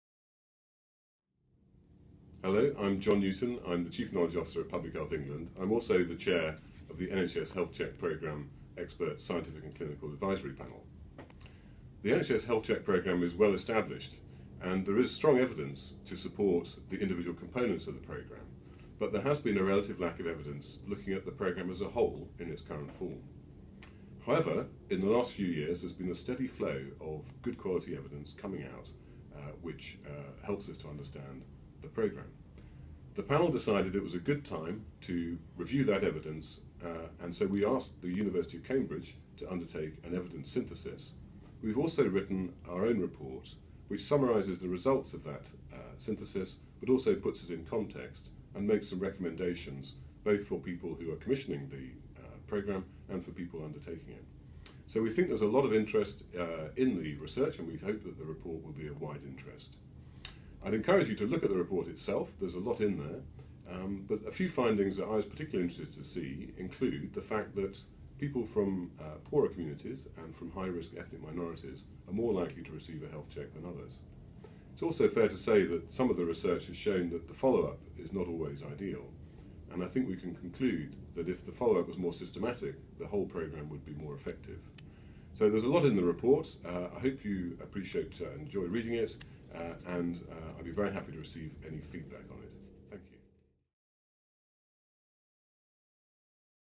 Emerging evidence on the NHS Health Check: findings and recommendations - John Newton
Professor John Newton, chair of the NHS Health Check Expert Scientific and Clinical Advisory Panel, provides an introduction to the Panel’s first publication reviewing the emerging evidence.